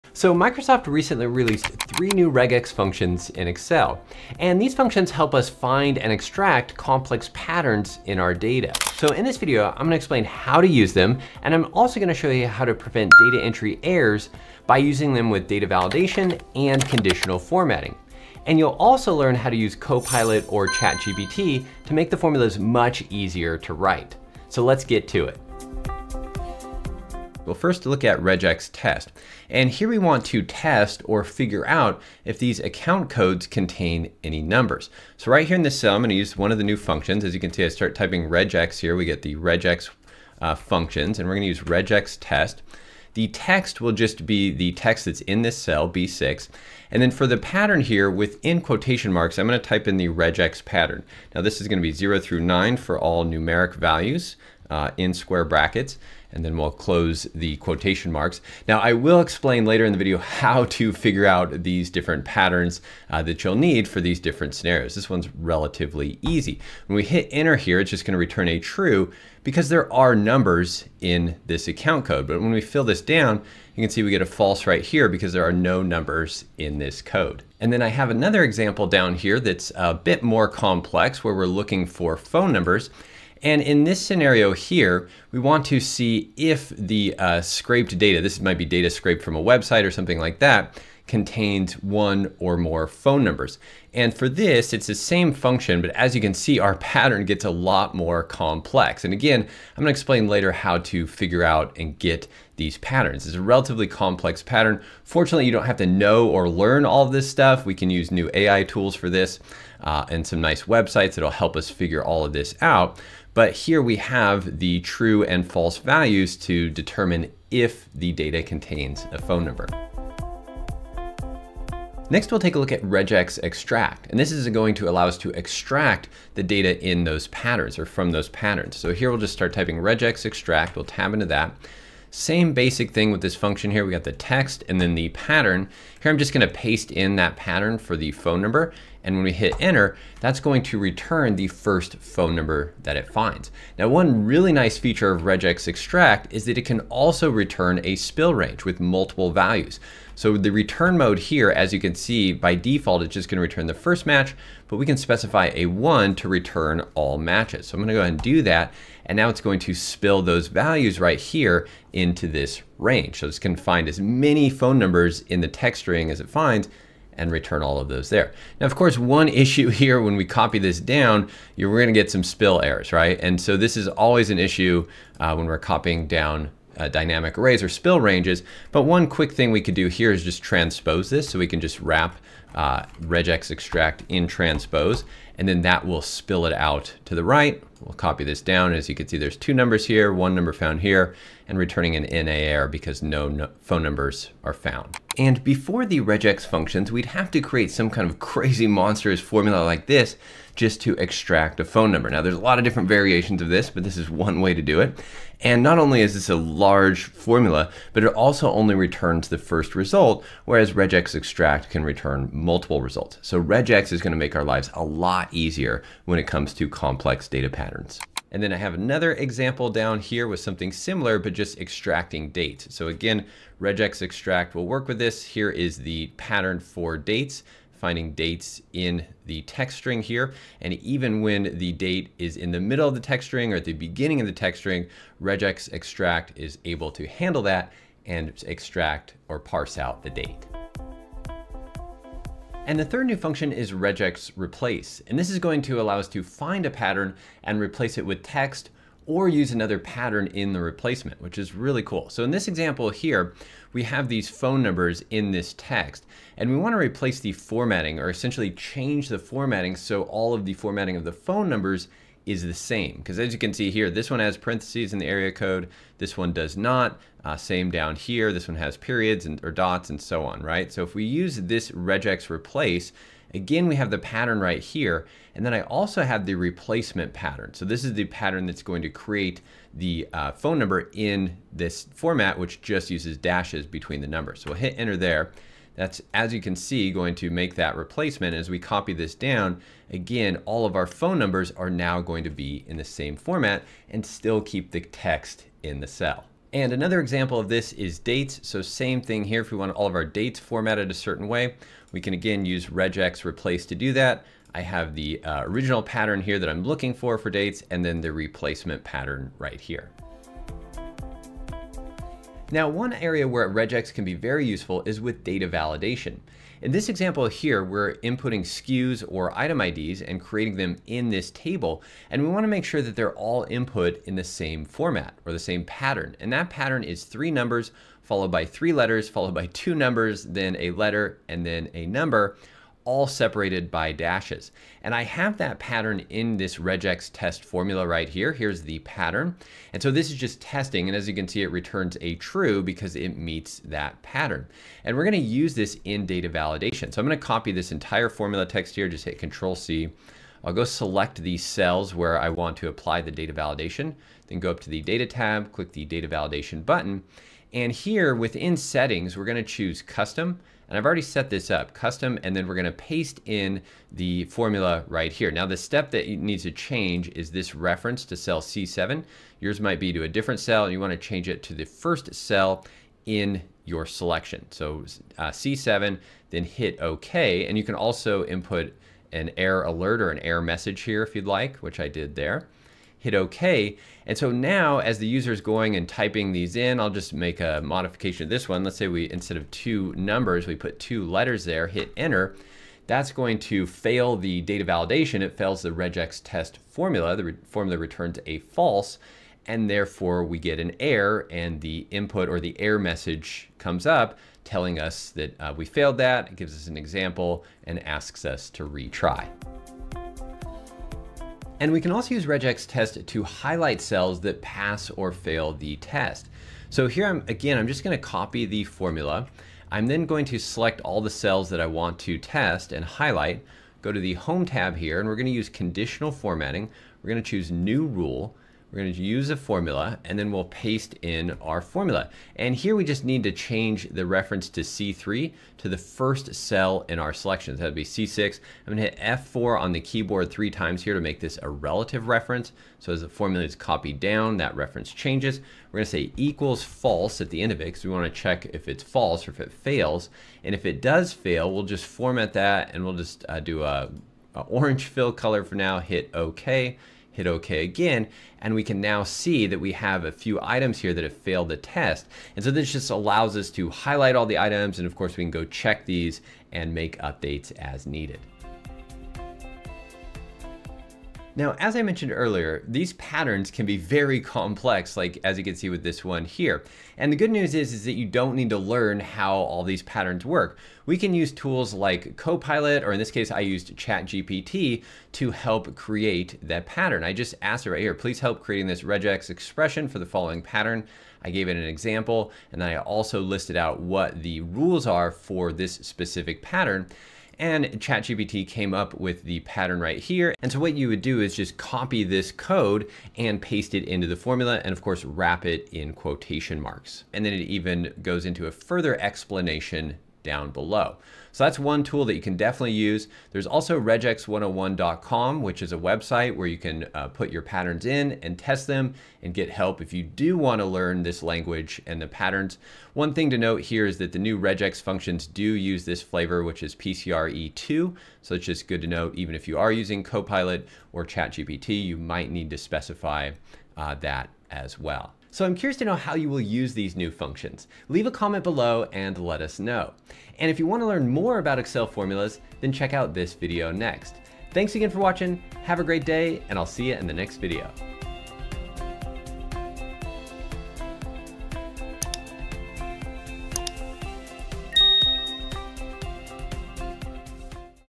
Easy Data Cleaning with NEW Regex Functions in Excel
Discover the magic of Excel Regex Functions! Learn how to use Regular Expressions to streamline data cleaning, extract specific information, and easily perform complex text manipulations. This tutorial will show you how to harness the full potential of Regex in Excel.

NOTE: At the time of this video being published, the 3 new REGEX functions are in beta and only available on the Beta Channel of Microsoft 365 for Windows or Mac. They are only available to a portion of users on the beta channel.

🔗LINKS

00:00 Introduction
00:26 REGEXTEST
02:00 REGEXEXTRACT
04:00 REGEXREPLACE
05:10 REGEX with Data Validation
06:50 REGEX with Conditional Formatting
07:50 Getting help with REGEX